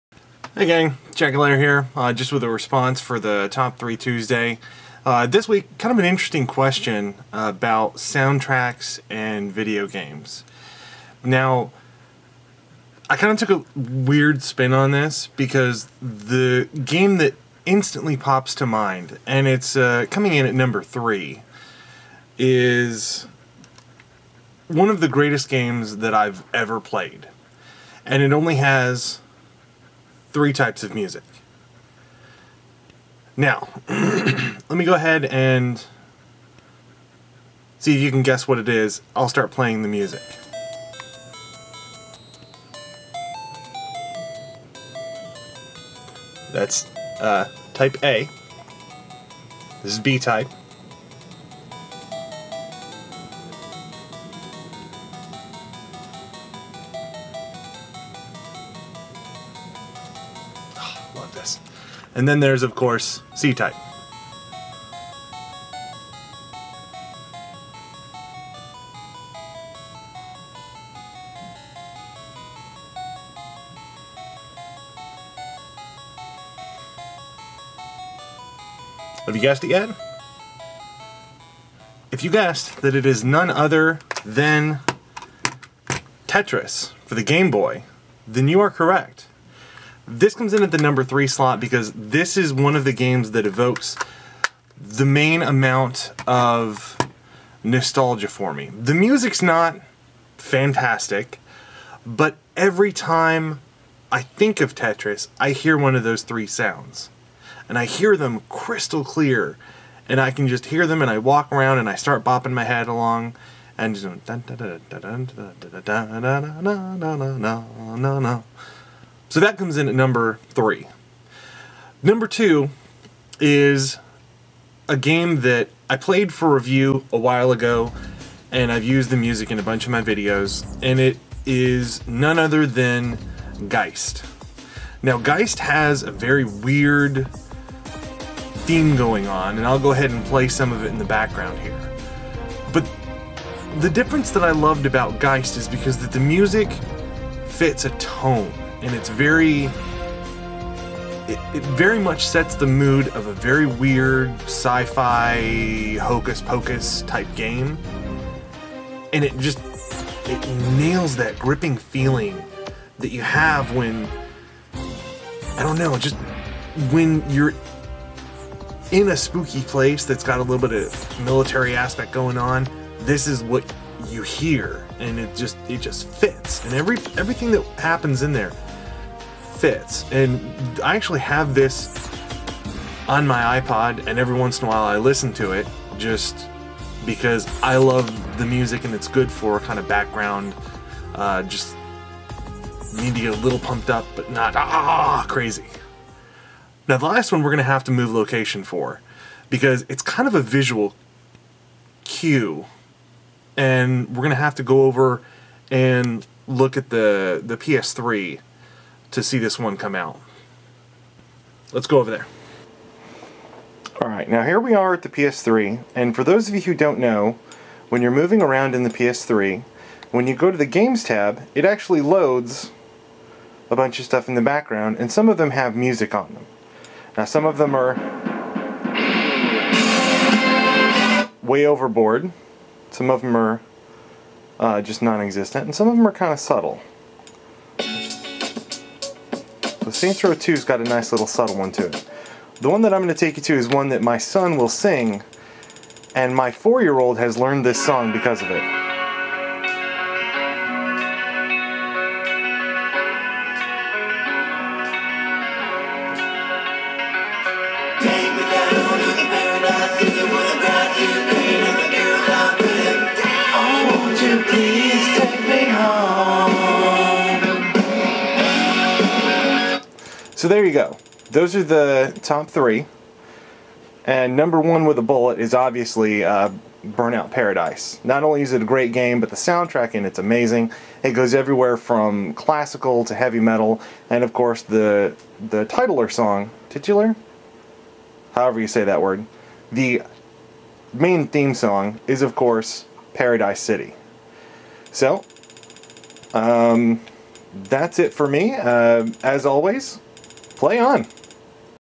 T3T Response Video: Favorite Video Game Music
A response to edt1138's Top 3 Tuesday video starring MichaelBTheGameGenie.

These are games that jump out at me musically, and they cross over 3 decades of video games.